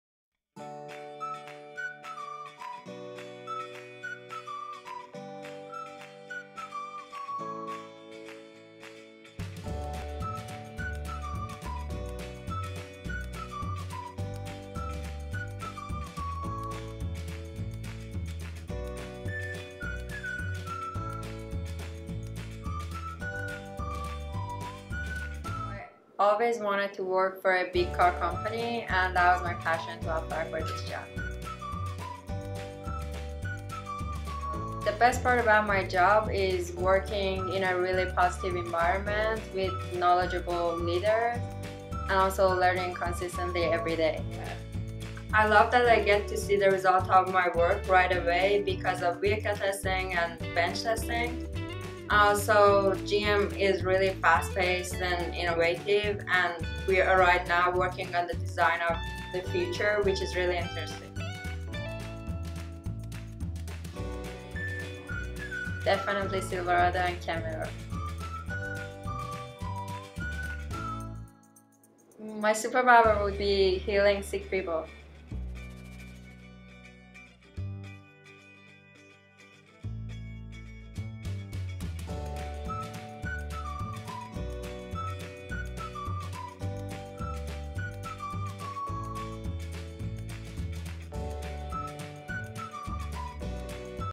Behind the scenes with Zahra Haeri, General Motors Controls System Engineer
Meet Zahra Haeri, Controls System Engineer at GM Canada. Zahra moved from British Columbia to Markham, Ontario in September 2017 to begin her career at GM and she is loving every second of it!